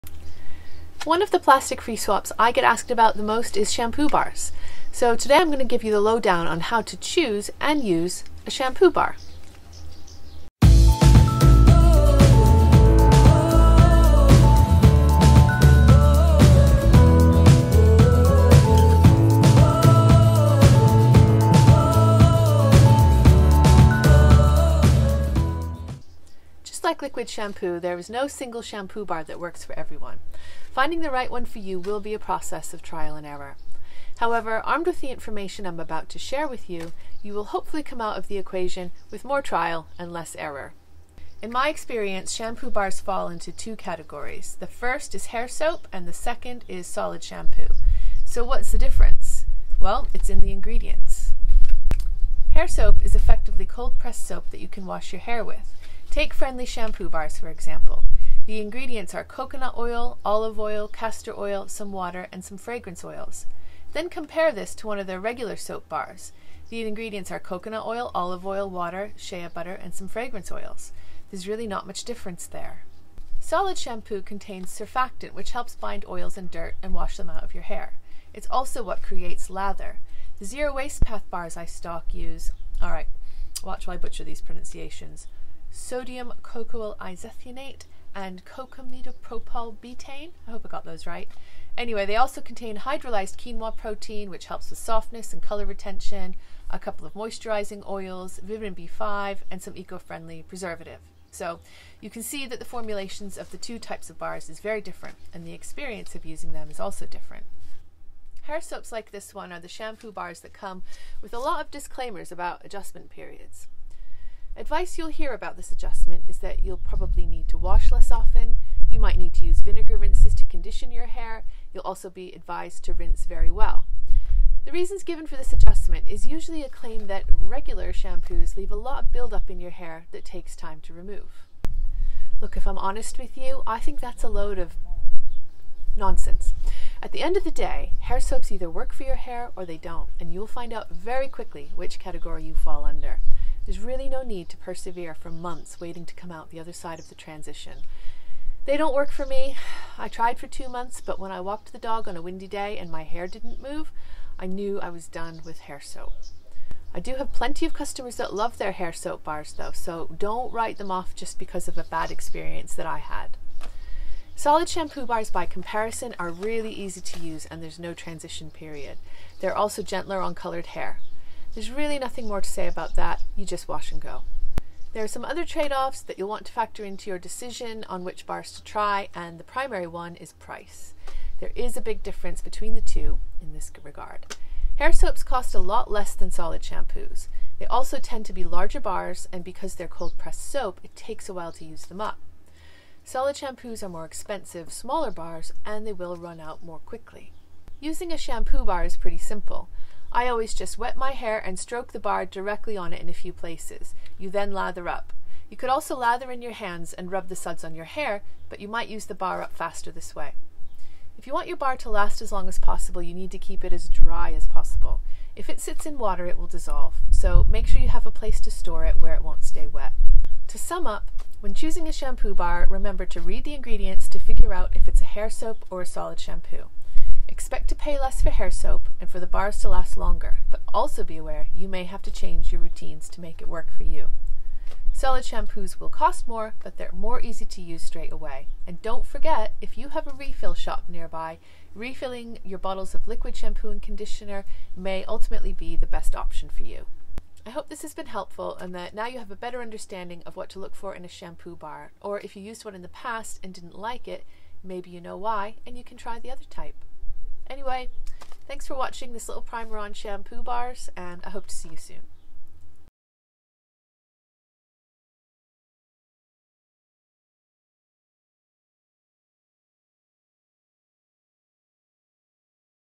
My shampoo bar nearly ruined my hair!
Are you going plastic free and you want to try a shampoo bar? Before you do, watch this video so you can make the best choice for you!

The shampoo bars mentioned in this video were by Friendly Soap and Zero Waste Path. They are both made in the UK, vegan, plastic free, cruelty free, and don't contain palm oil.

Refills on the Road is driving Zero Waste in Derbyshire, UK